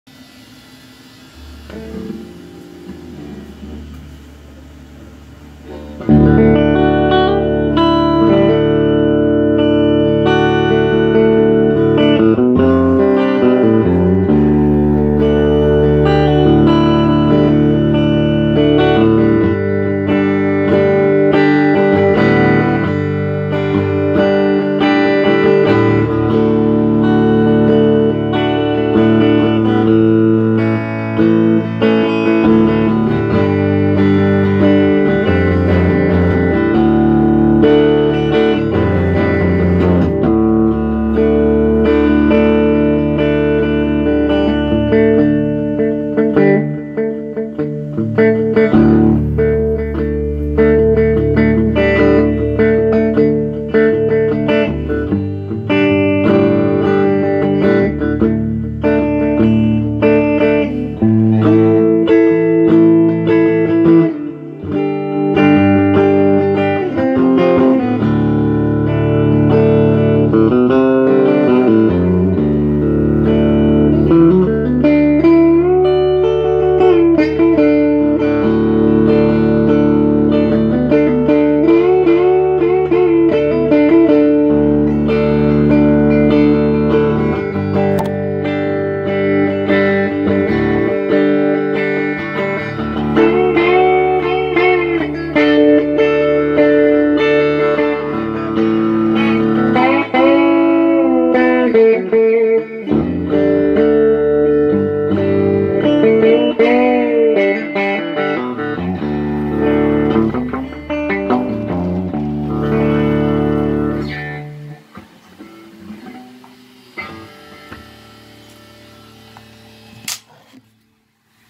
1954 Les Paul,1960 Bassman, ThroBak Round core strings
Yes my camera flops, but I was amazed on how good Round Core Nickle wound ThroBak strings sounded. Jon great job. Note9 on top of amp probably not the best choice but the recording sounded pretty clear.  Yes I could do many takes to get the best playing,  recording etc, video editing etc... maybe someday.